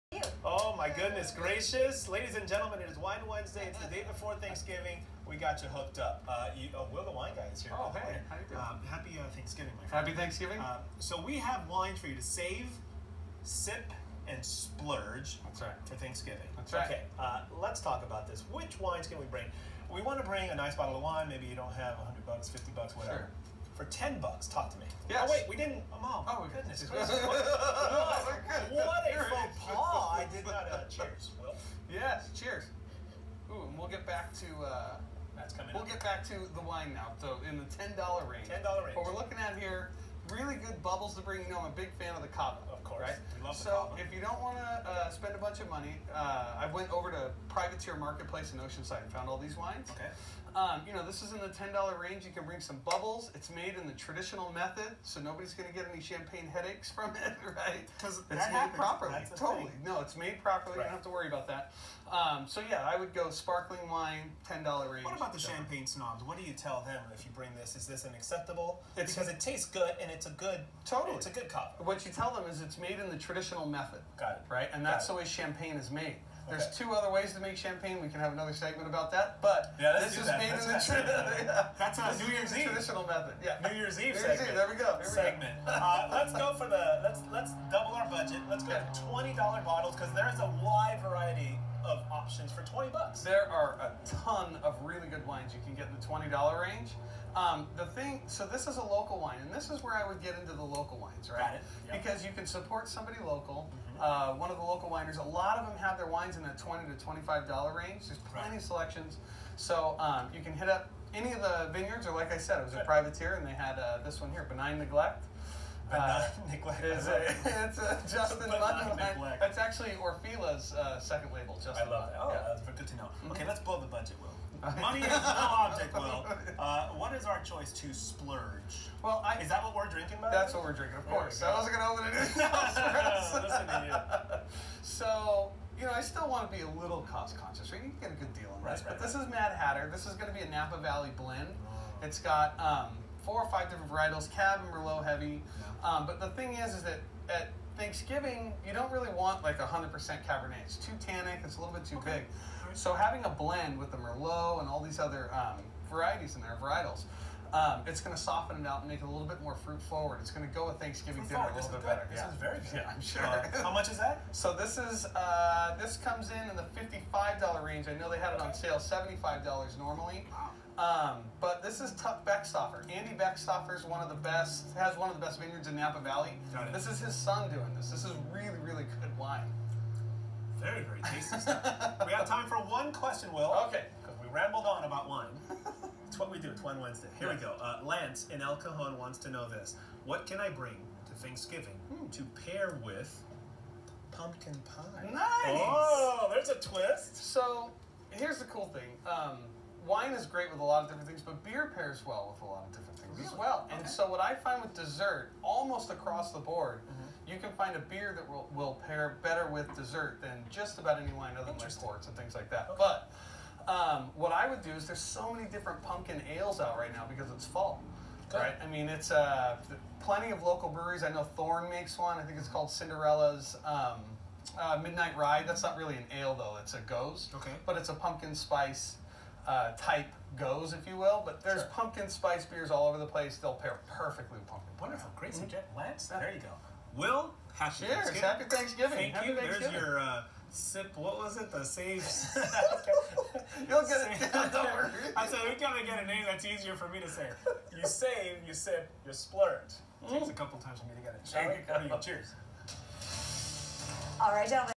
Skater Girl Bottle Fox 5 San Diego
Skater Girl prominently displayed on Fox 5 San Diego.